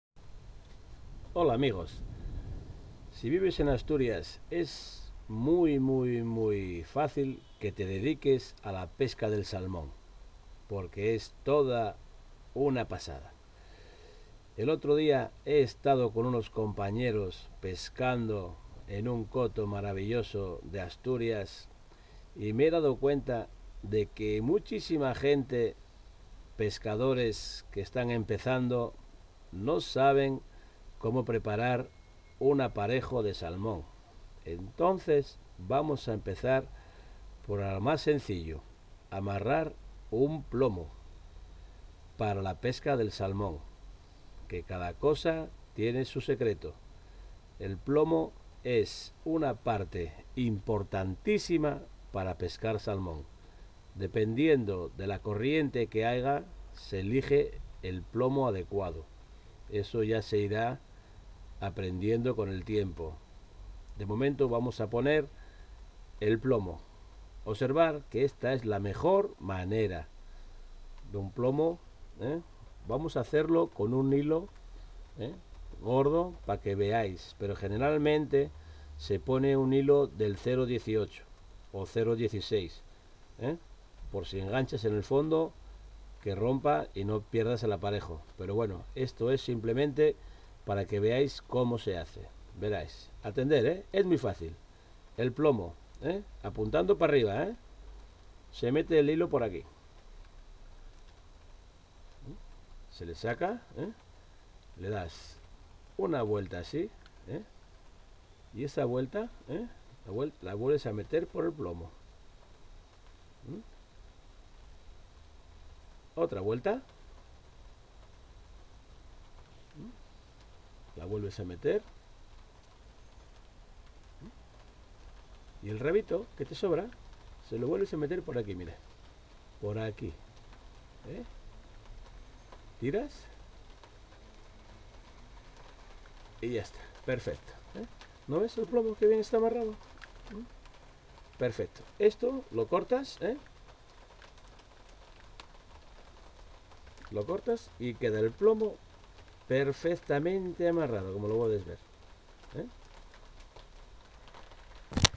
Nudo básico para atar "el plomo" pesca del Salmón,1º parte, con Tino Faez
Clases de pesca con tino Faez, "El plomo" mejor forma de atar un plomo para la pesca del Salmón, muy importante.